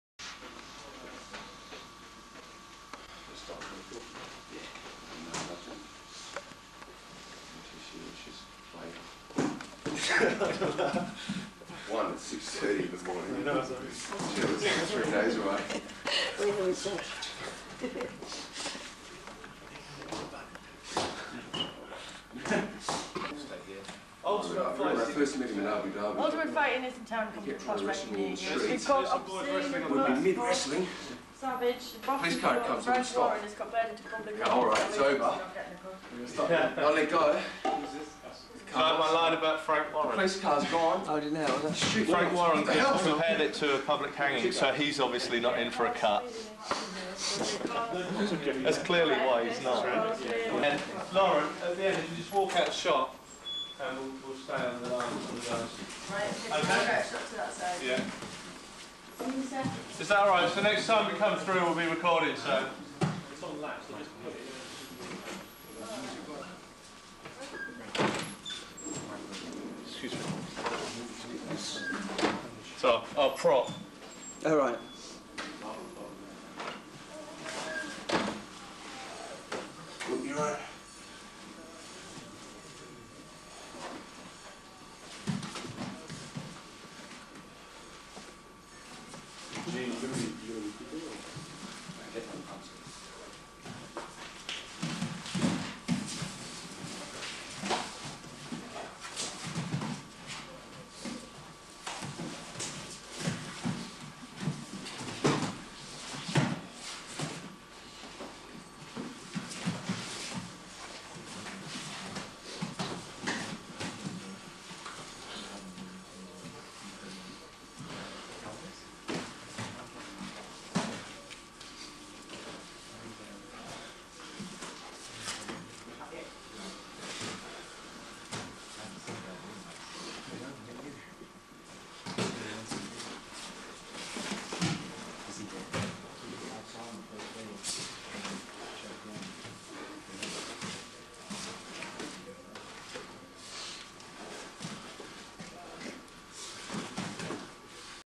Weir & Sinosic Training UFC 38.mpg
First ever Ultimate fighting championship in England. 2002 Both Mark Weir & Elvis Sinosic team up for all the interviews around the country